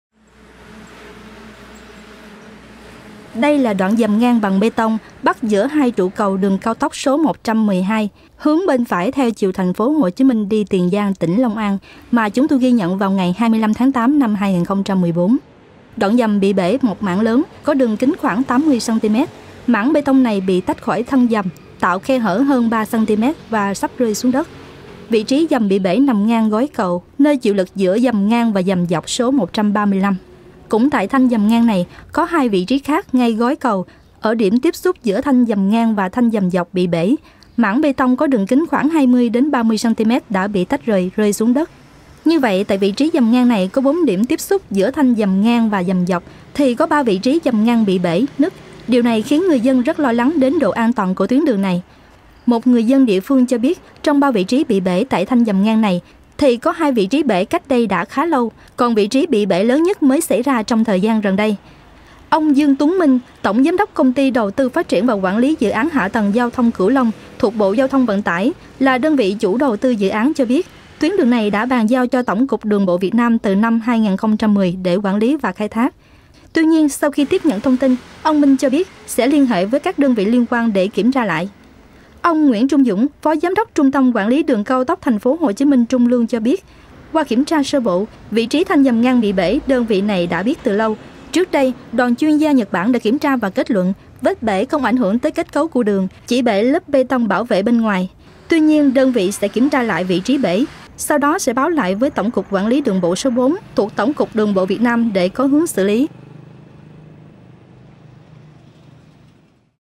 Đường Cao Tốc Tp.hcm – Trung Lương Bị Nứt Nghiêm Trọng
(Nguồn: Tuổi Trẻ) Một thanh dầm ngang của đường cao tốc TP.HCM – Trung Lương bị NỨT NGHIÊM TRỌNG